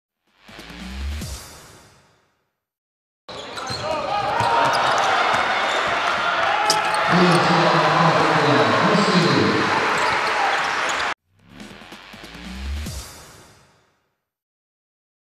Kostja Mushidi throws down a fancy dunk (Mega Bemax - Budućnost VOLI, 13.1.2018)
Kostja Mushidi makes a steal and rewards himself with a fancy dunk!